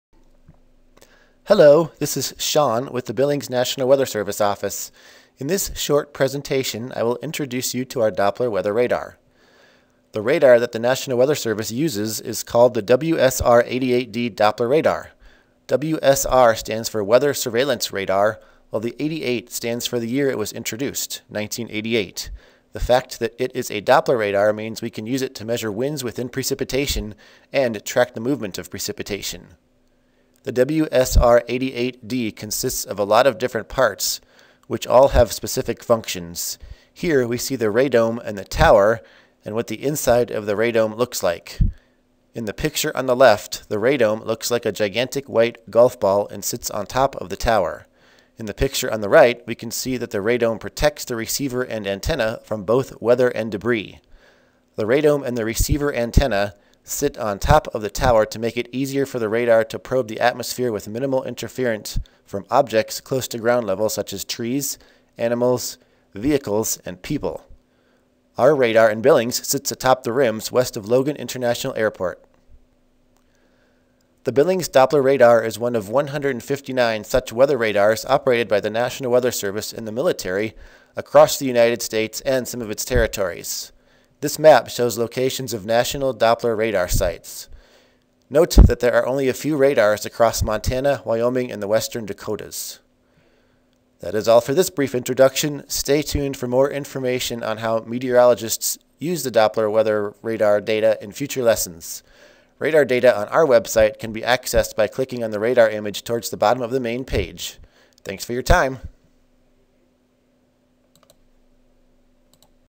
Doppler Weather Radar - an Introduction
This is a brief introduction to the WSR-88D Doppler Weather Radar used by NWS offices.